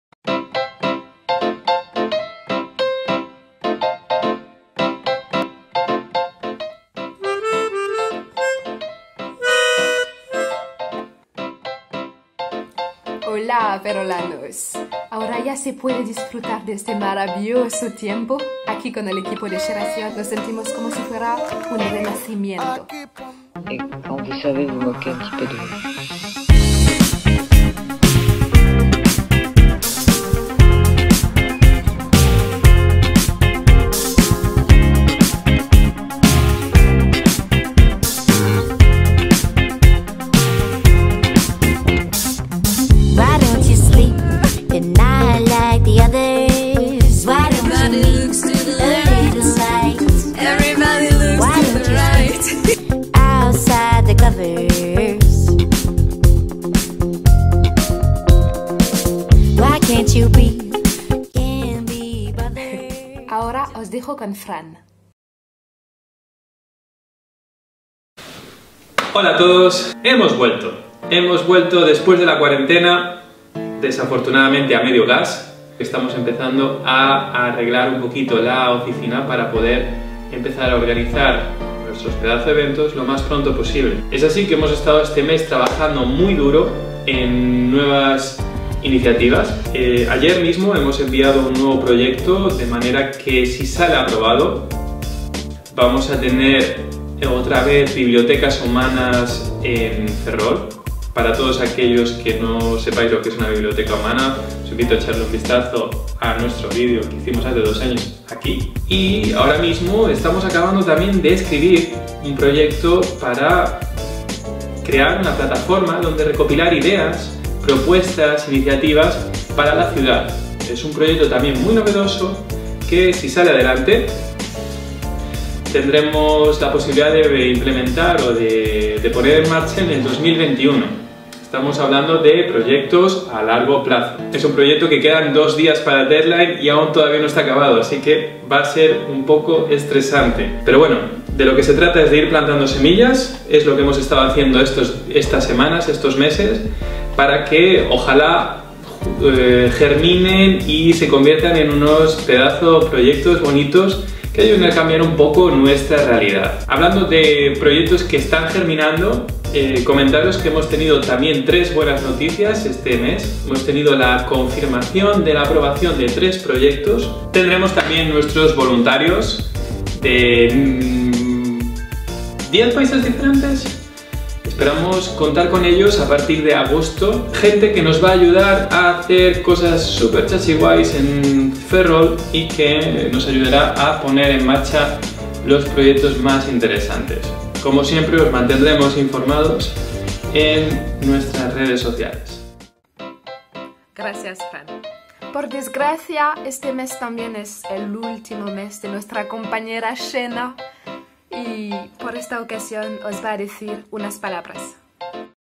Mayo - Hemos vuelto 😏
Le hemos sacado las telarañas a la oficina y hemos entrado en la nuevanormalidad con más ganas que nunca. Nuestra cocina de proyectos sigue echando humo, así que parece que vamos a tener un verano bastante movido.